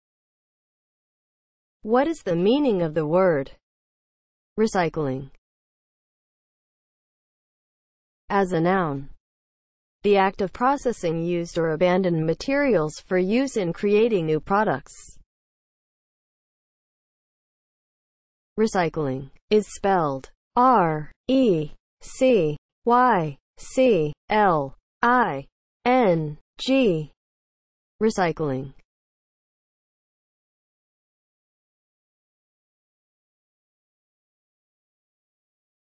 What is the meaning of the word RECYCLING?
Definition and spelling of the word RECYCLING.

(00:00) Intro
(00:07) As a noun
(00:17) Spelling

As a noun (00:07):
• the act of processing used or abandoned materials for use in creating new products